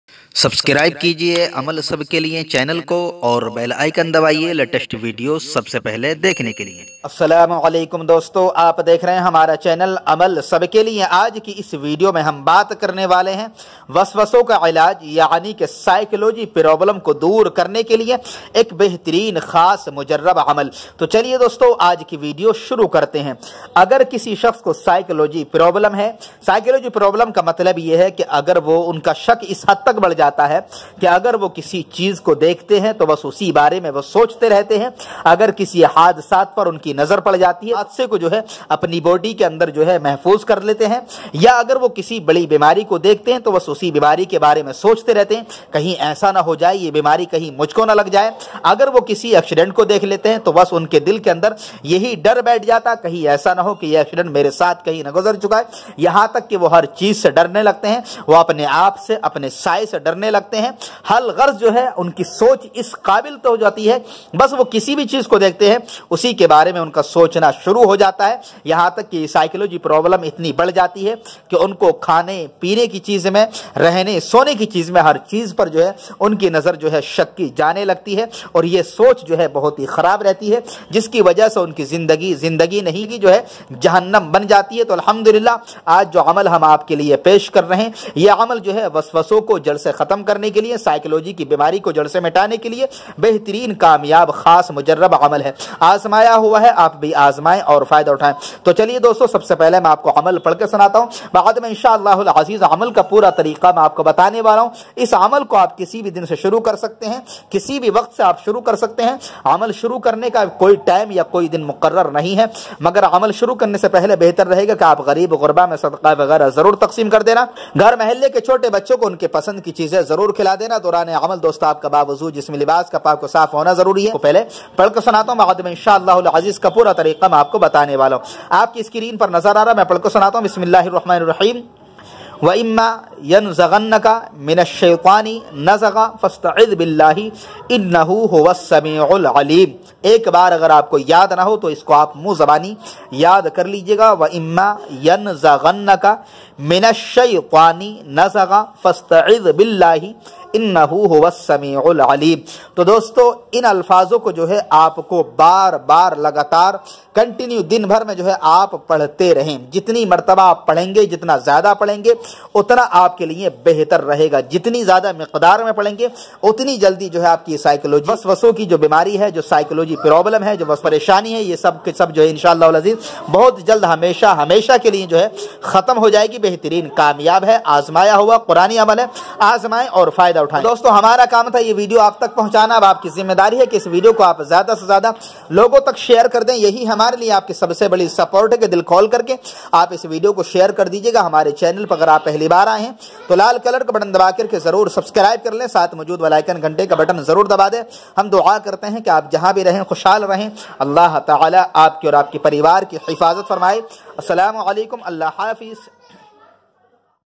Mental Problem Solution | Psychology Problem | وسوسوں کا علاج
welcome to your YouTube channel Amal Sabke Liye

herbivores aaj kisi video mein humne psychology problem ka ilaj bataye hai agar kisi Ko waswaso ki Bimari hai mentality problem hai to ye asan wazifa kamyab hai psychology was vasuki problems ko khatam karne ke liye

About: Me Molana Muhammad Furqan Raza Qadri...